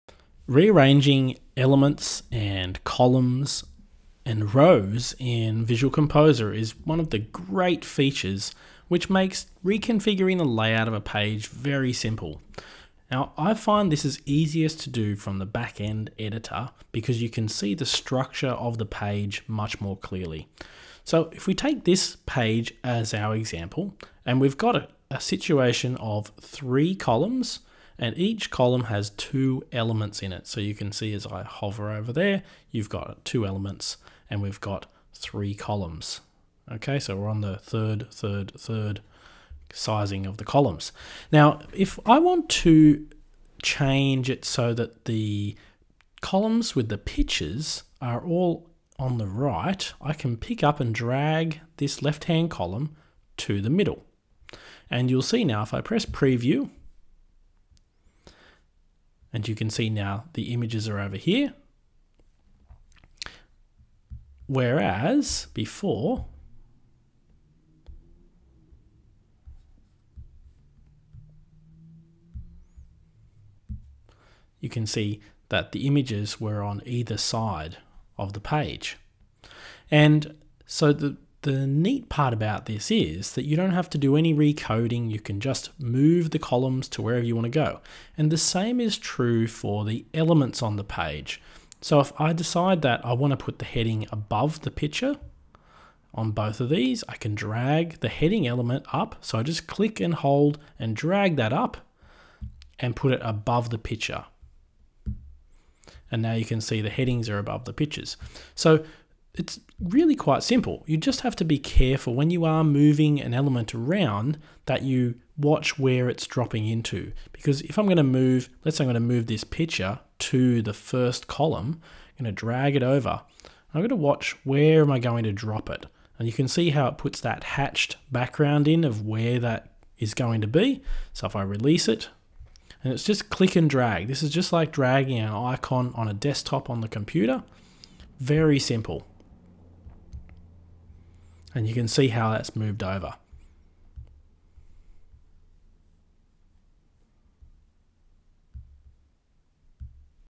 Rearrange columns and elements in visual composer
An easy to use content editor for WordPress. This WordPress tutorial video shows how you can drag and drop elements within a page.